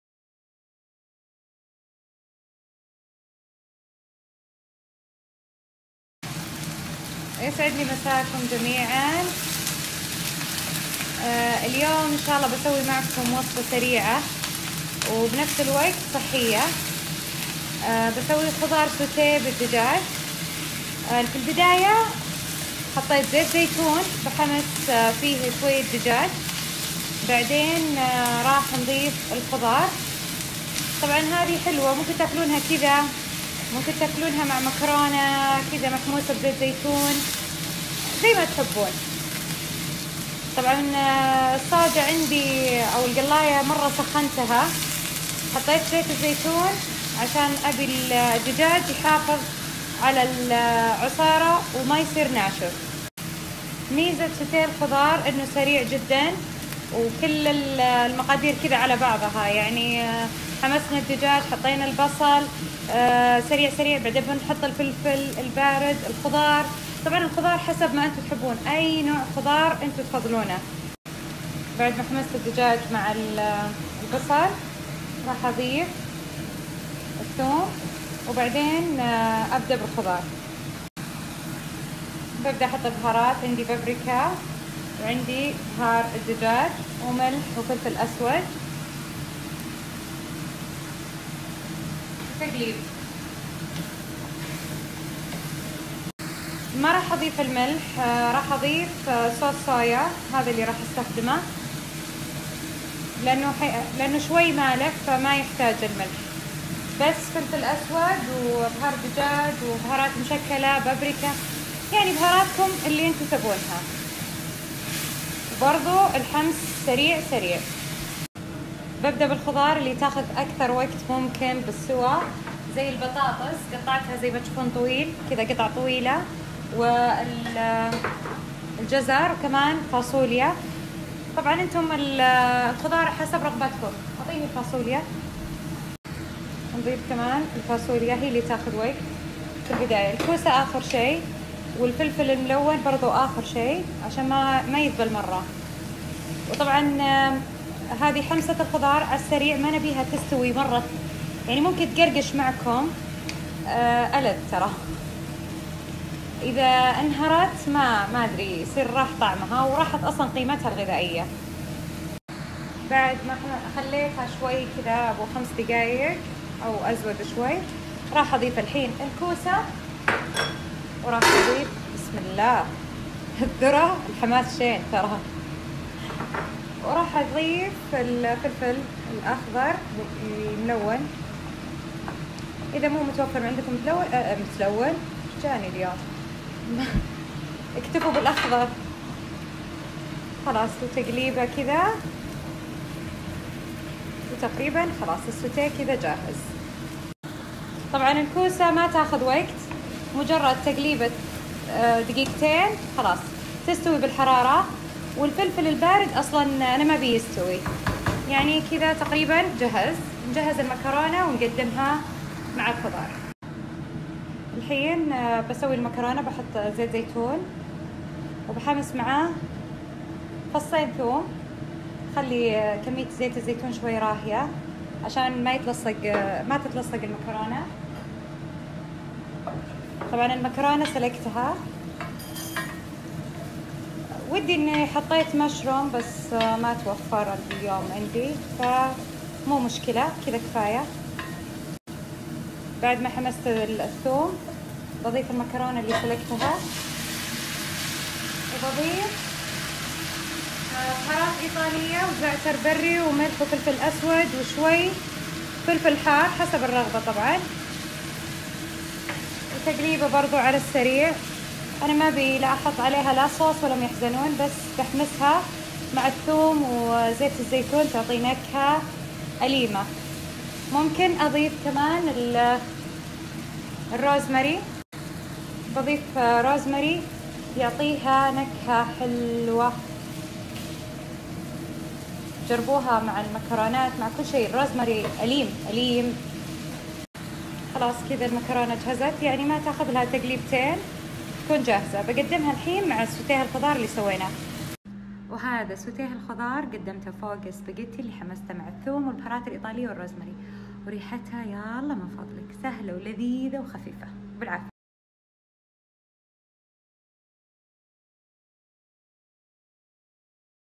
سوتيه الخضار مع الدجاج والسباقيتي لذييييييذ 😍👌الطريقة مكتوبة بصندوق الوصف🤓👇
.المقادير:
دجاج صدور مقطعة بشكل طولي
بصل ابيض مقطع شرايح  وثوم
فلفل ملون مقطع شرايح
واي خضار عندك مقطعة طويل انا اللي اضفته
كوسة  وجزر وفاصوليا وبطاطس مقطع طوال
اذا عندك بروكلي ياسلااام بتطلع لذيذة بعد

صويا صوص او تتبيلة ماجي
بهار مشكل او بهار الخضار من ماجيك
مااحتاجت ملح لان الصويا مالحة

الطريقة :
يحمس الدجاج وبعده البصل ونصف كمية الفلفل الملون مع شوي زيت زيتون وبعدها اضفت الثوم  ثم اضفت الفاصوليا والجزر وحمستهم زين بدون اضافة موية فقط اضفت الصويا صوص
وعقبها ضفت االبطاطس وحمستها لمدة عشر دقايق واضفت لك مقدار بيالة موية وخليته ثلاث دقايق بعدين اضفت الكوسة  وحمسة ع السريع وغطيته لمدة خمس دقايق  بس  وقلبته تقليبة وغرفته لانه لازم ماتكون الخضار منهرية او مستوية مره لان سوتيه الخضار يكون نصف سواء الذ
وتقدم مع مكرونة مسلوقة ومحموسة بشوي زيت او زبدة وزعتر بري او بهارات ايطالية
او تقدم مع رز ابيض او مع الخبز وبالعاااافية ❤️